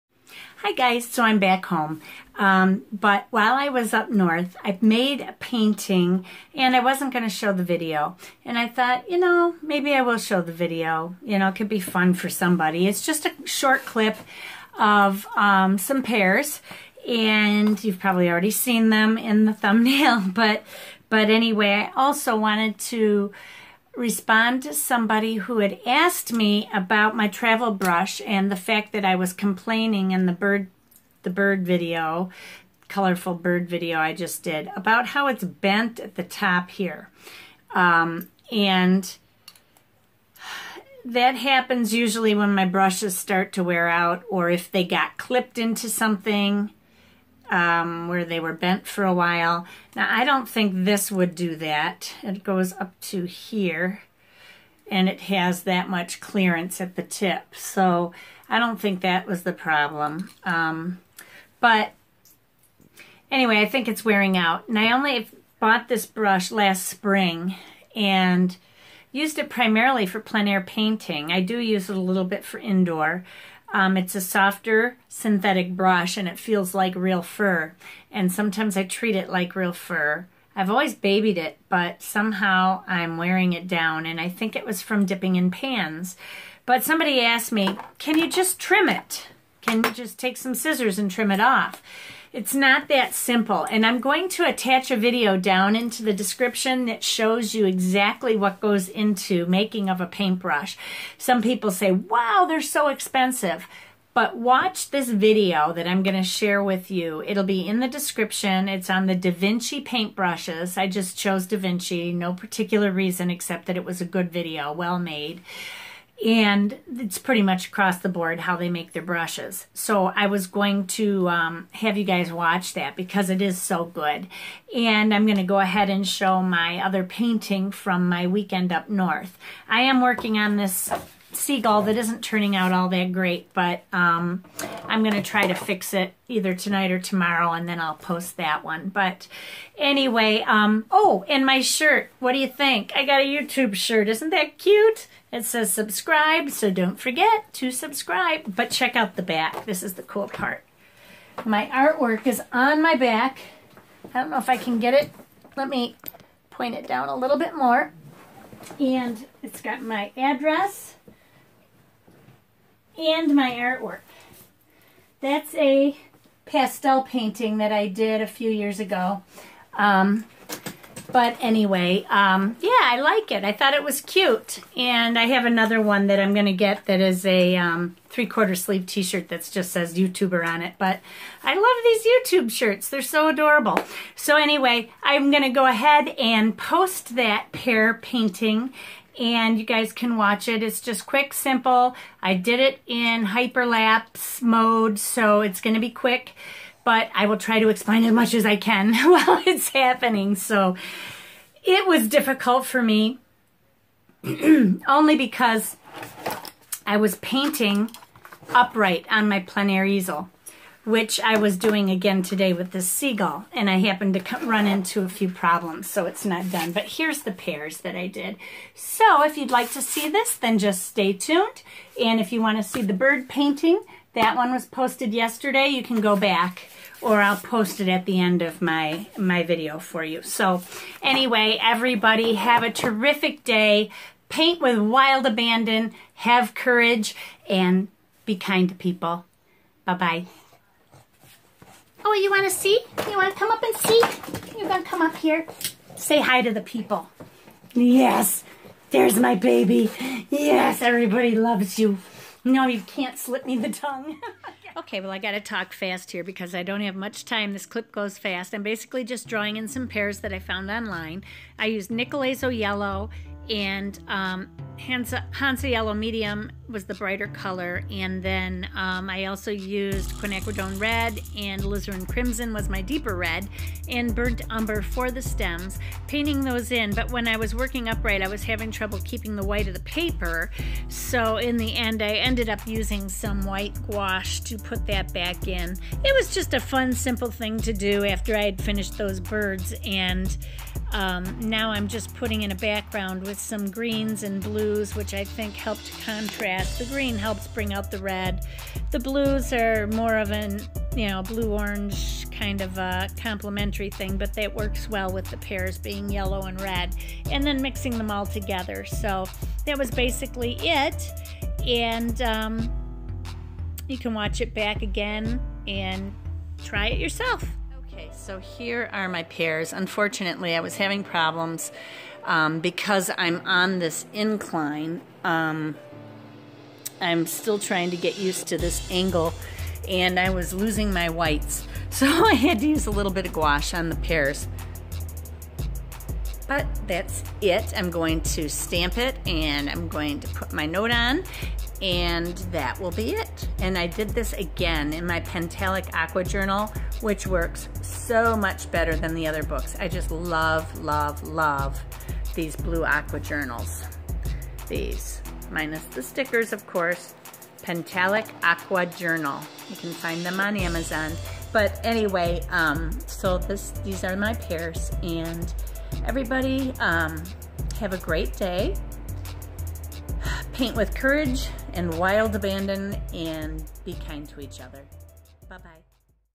Pears Watercolor Painting
Using Daniel Smith watercolors primarily and my Pentalic Aqua Journal 8.5"x11"

Instagram Sharon Cullen Art

Google+ Sharon Cullen Art